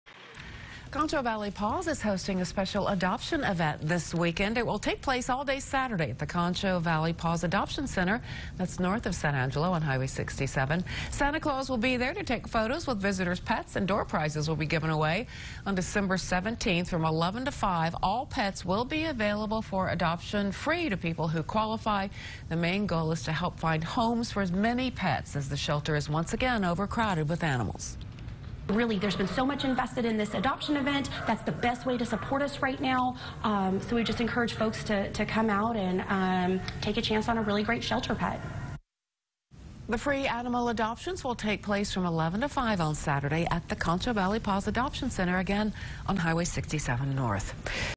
"Paw"liday event for the holidays
Concho Valley PAWS is hosting a free adoption event on Saturday, Dec. 17.